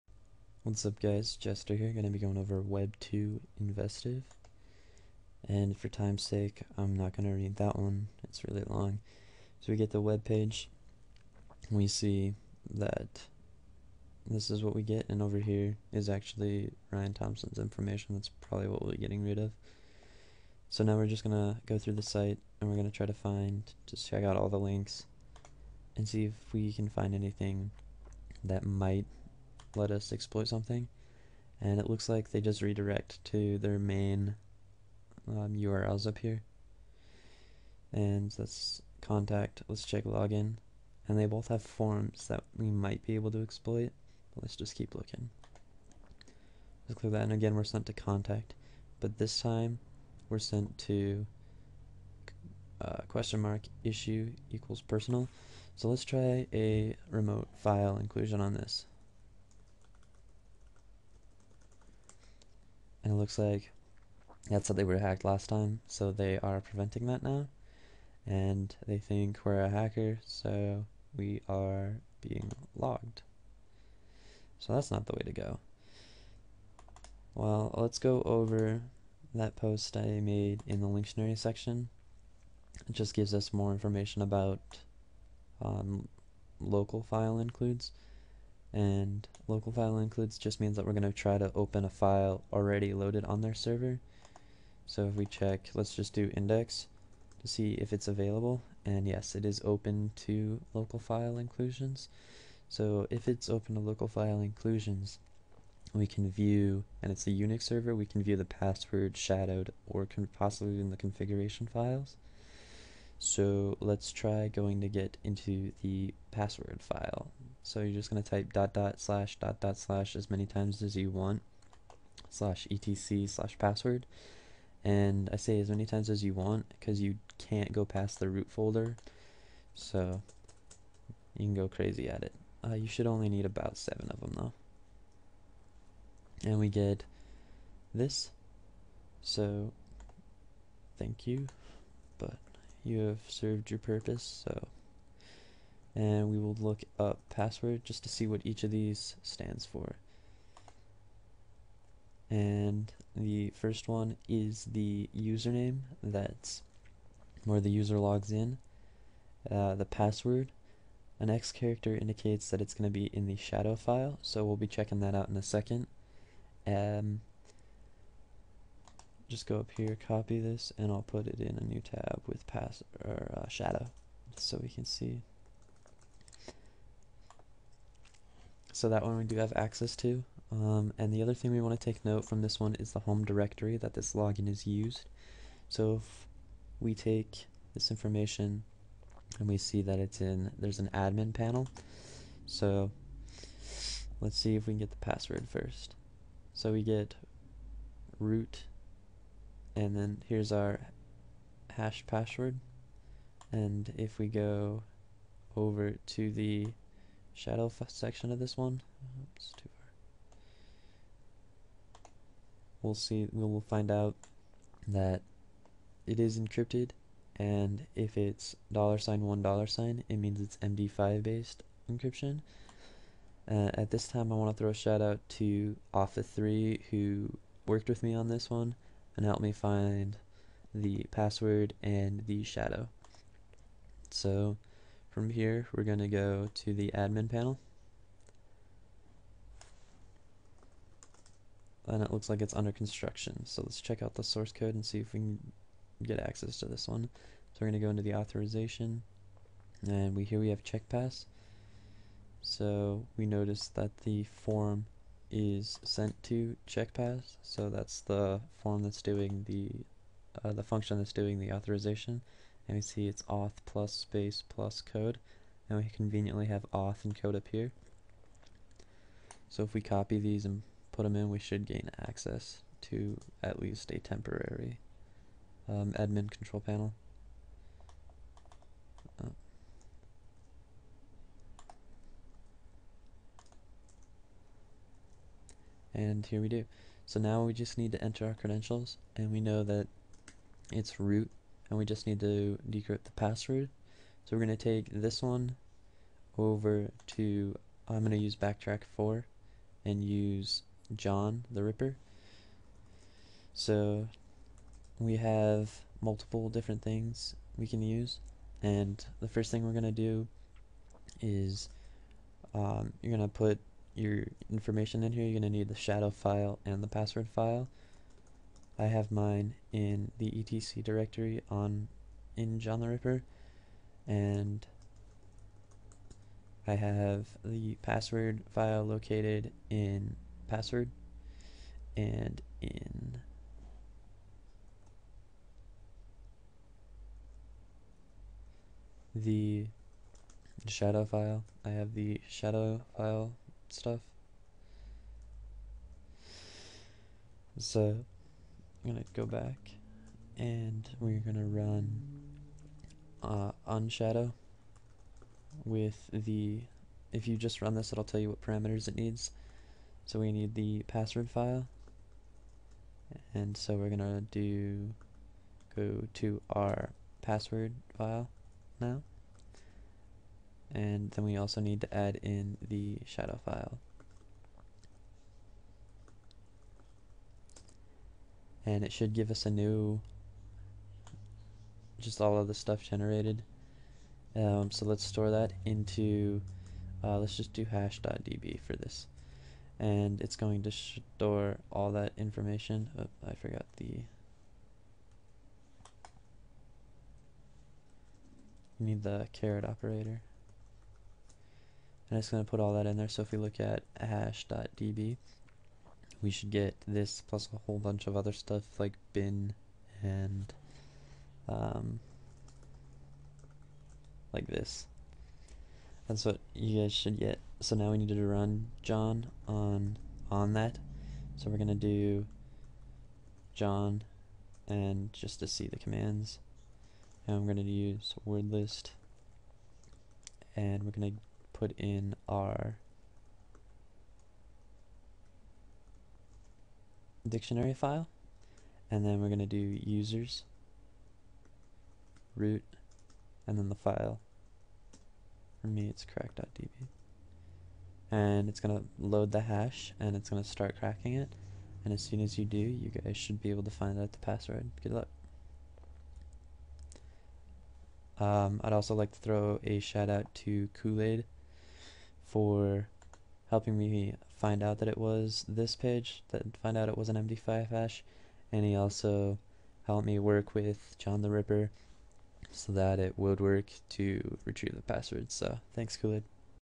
HaxMe Web Mission 2
Thanks to Awfa3 and KoolAide for helping me out with this one!
Spoiler-free walkthrough of completing this mission. This is best used as a supplement. Ergo you don't just watch it to see how it's done but attempt to get as far as you can then if you get stuck look at this to push you in the right direction.